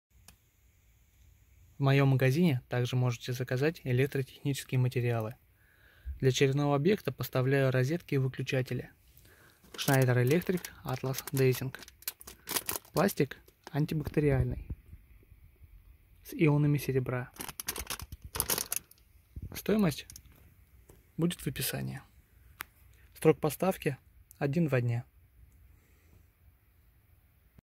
Atlasdesing розетки и выключатели
В моем магазине вы также можете заказать электротехнические материалы. Для очередного обьекта поставляю розетки и выключатели от компании шнайдер электрик серии атлас дейзинг. Термопласт: антибактериальный, с ионами серебра. Качественные материалы по доступной цене.

- выключатель 1-кл, 153 р
- выключатель 2-кл, 199.8 р
- розетка с заземлением, 166.2 р
- рамка 1п, 50.4 р
- рамка 2п, 82.8 р